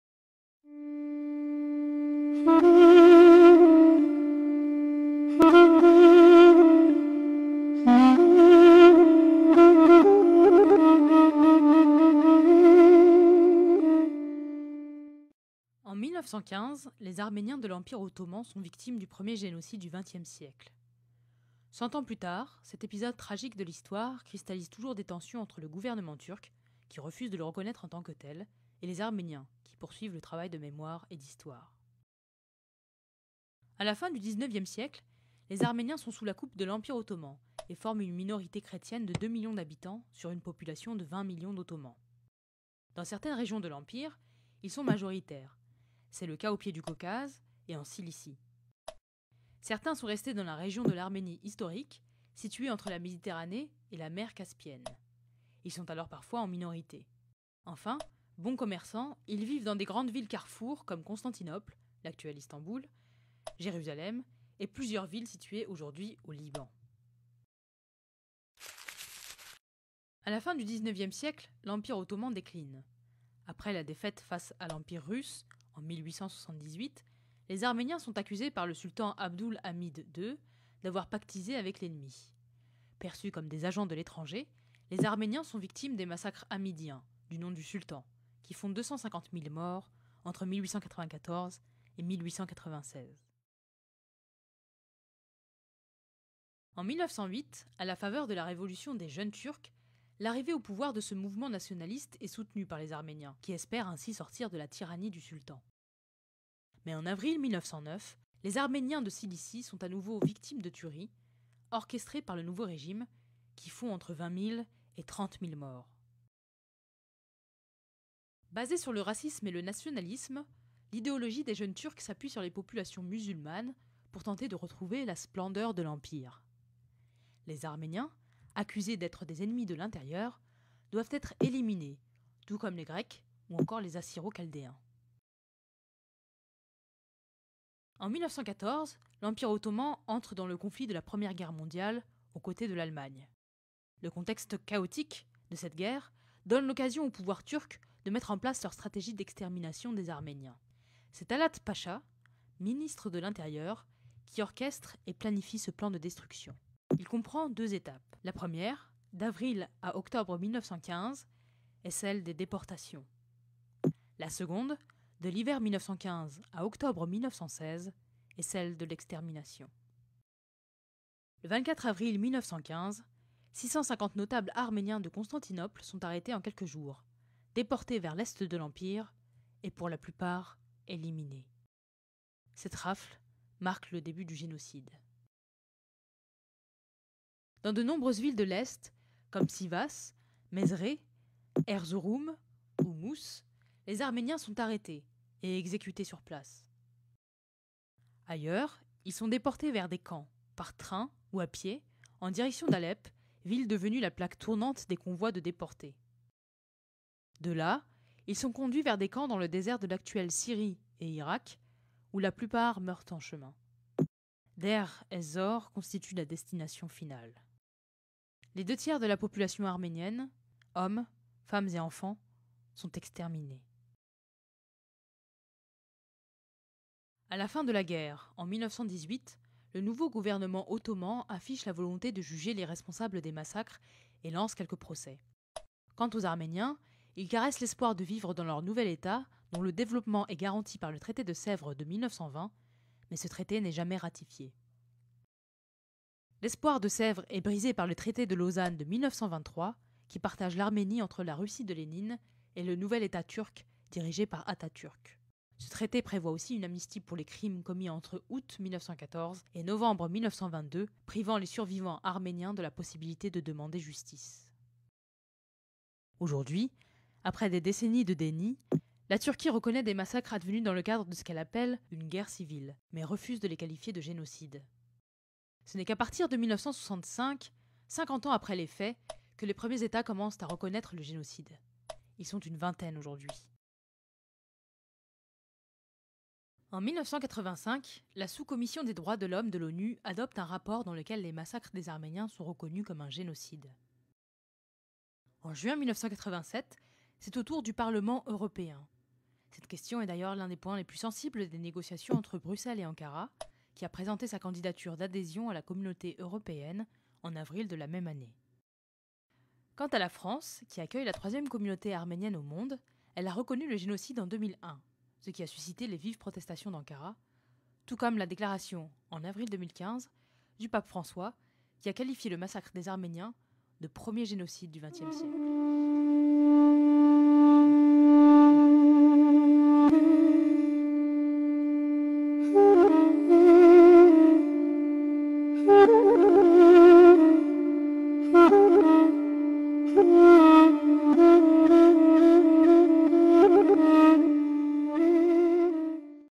CLG Lamure Génocide Arménien
3e CLG Lamure - HISTOIRE
Le Génocide arménien de 1915
Capsule vidéo utilisée dans le cadre de la ClasseInversée et de la réalisation d'un QCM en ligne